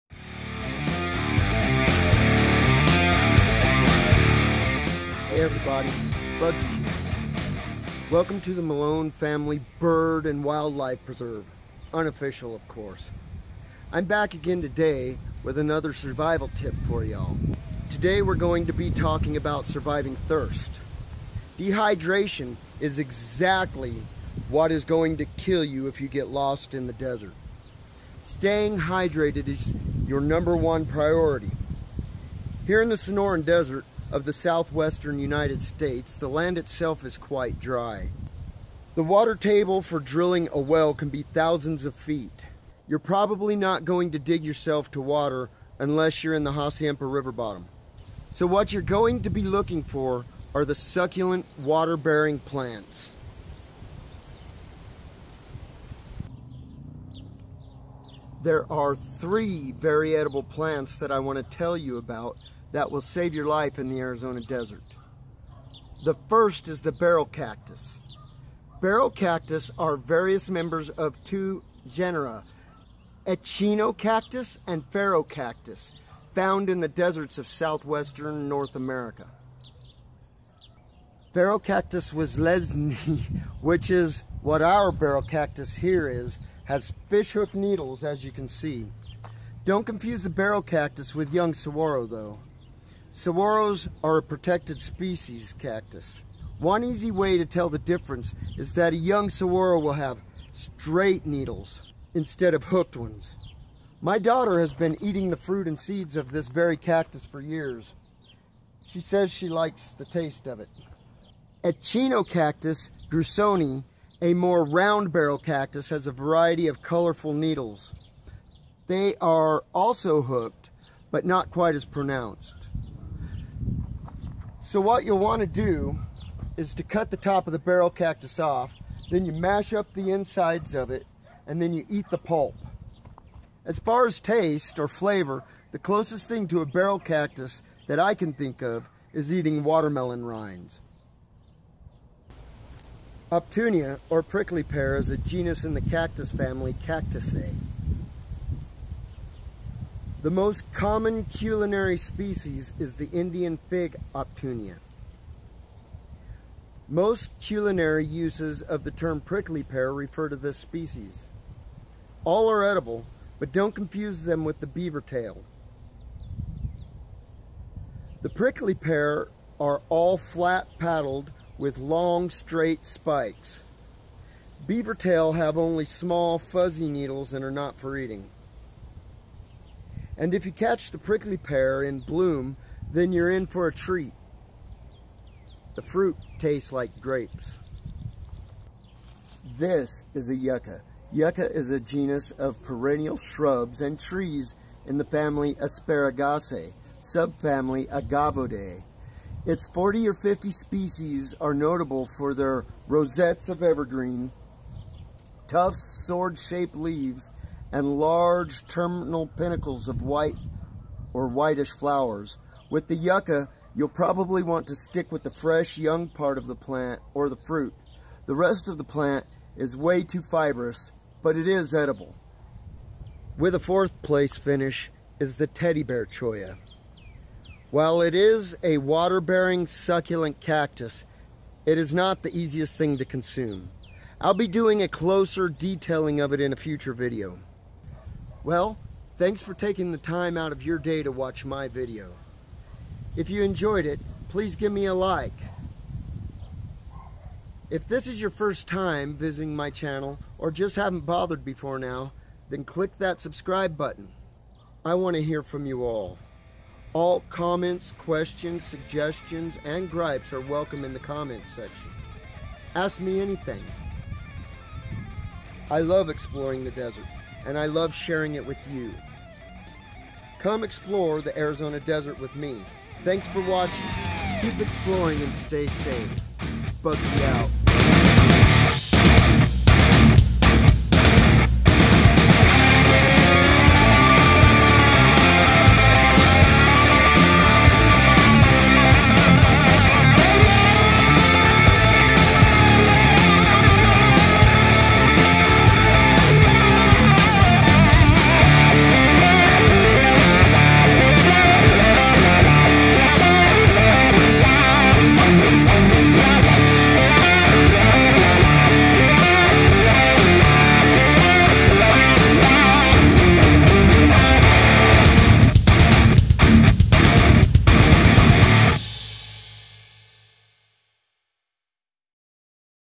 Bugzy's Survival Tips #2 - Surviving Thirst
Bugzy's Survival Tips
Surviving Thirst

Let me introduce you to
three very edible plants,
that grow wild here in the
Sonoran Desert, of the
Southwestern United States.

Do not die of thirst in the
desert! Know where to find
water!